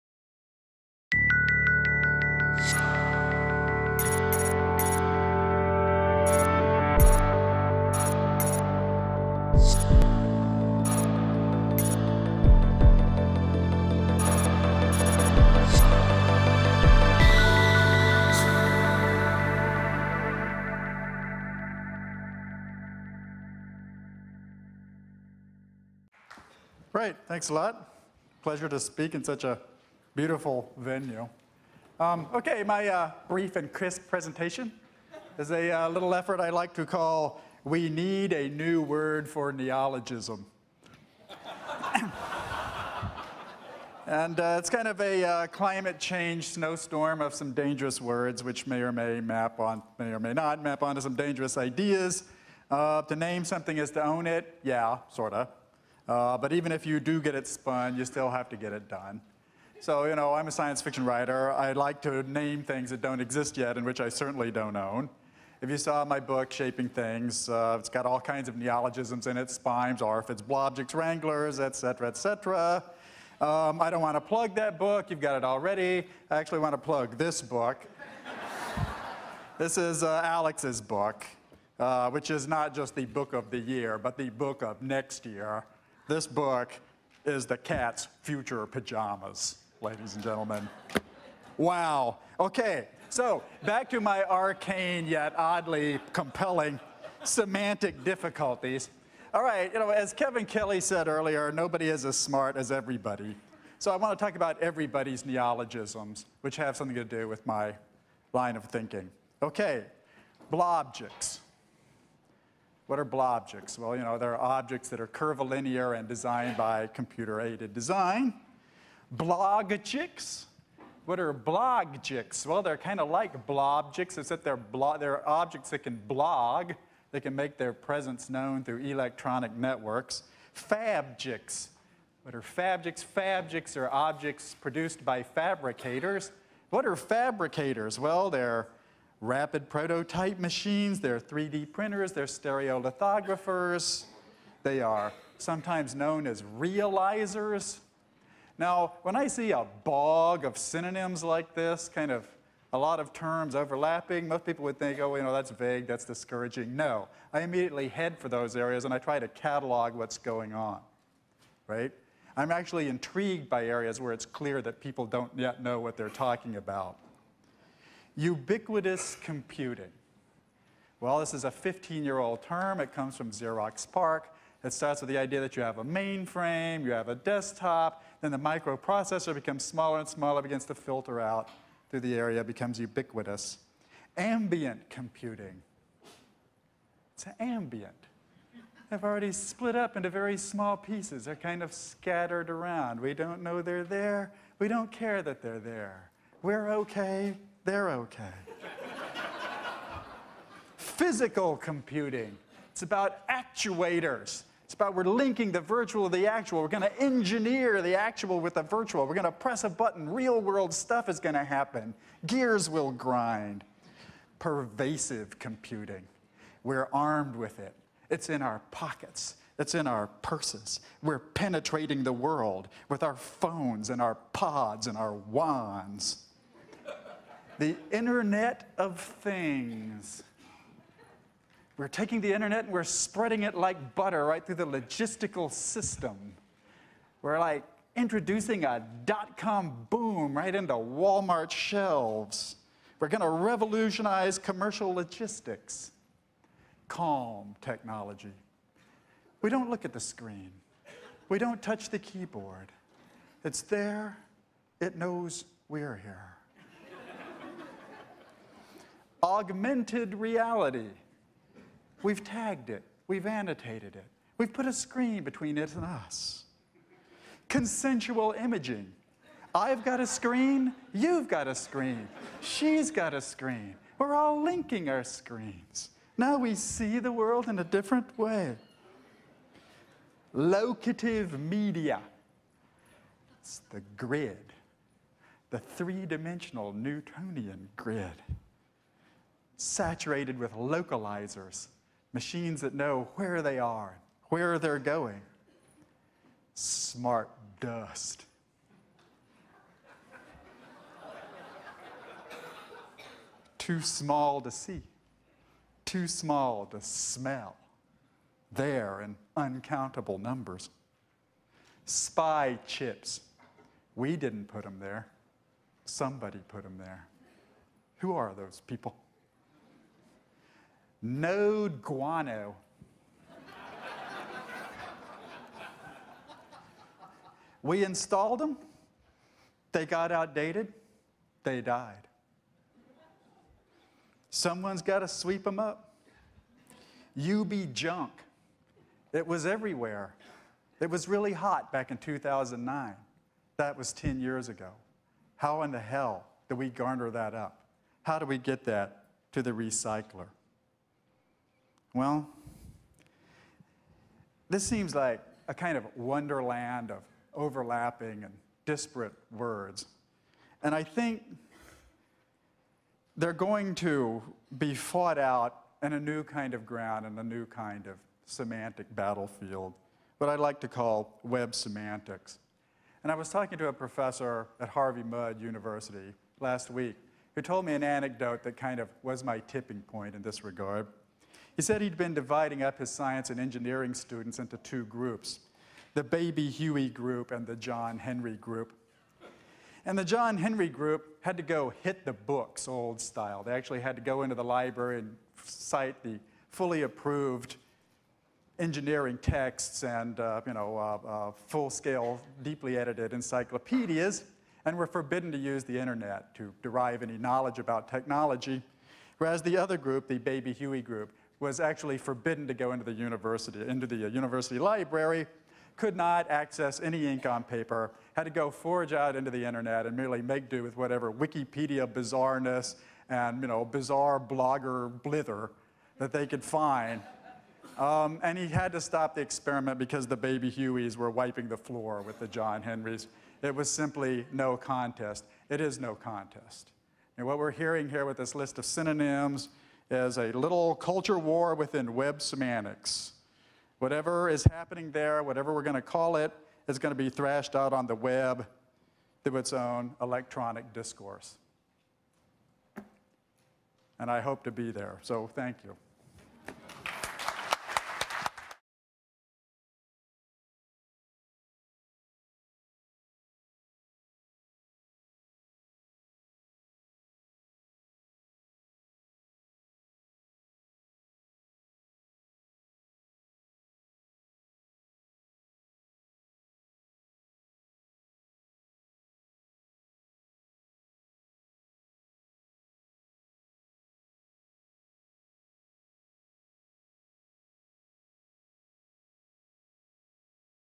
Bruce Sterling - POPTECH 2006
Author, journalist and contributing editor at Wired magazine Bruce Sterling understands why people get confused about new technology concepts. In what he sees as a culture war of web semantics, Bruce gets the audience’s attention with a unique call for a new vocabulary to better describe experiences with technology.